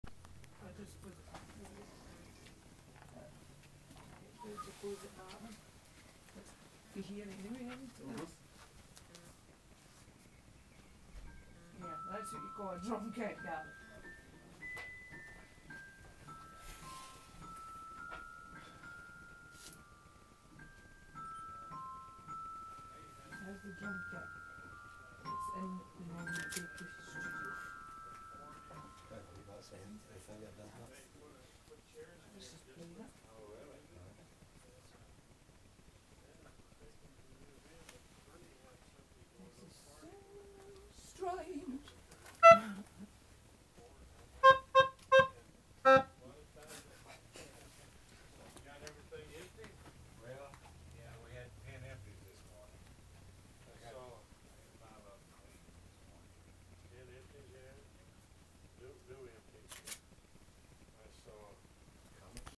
Buddy Holly. Norman Petty Studio 2.
Inside Norman Petty's Studio, Clovis, New Mexico. This is the very studio where Buddy Holly recorded many of his biggest hits. All the instruments are originals.